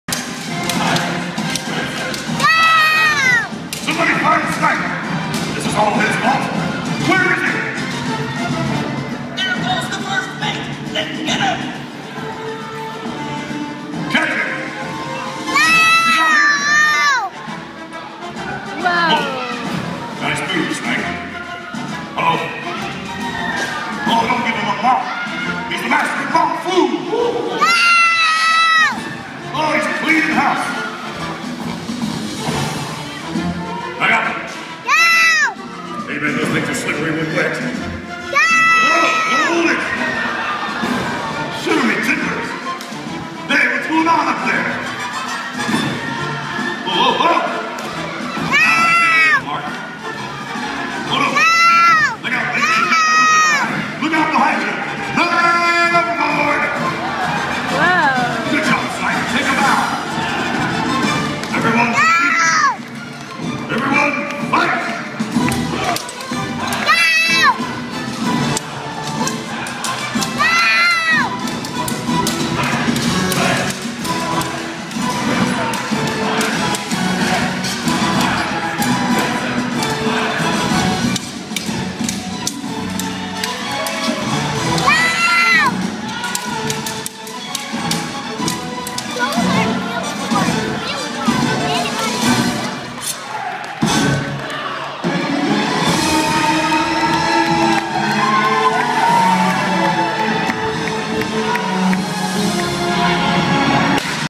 A Pirate's Voyage - Crimson vs. Sapphire
At the end of the show, the two pirate teams battle it out for treasure. Lilah got so into it as she cheered her red team on.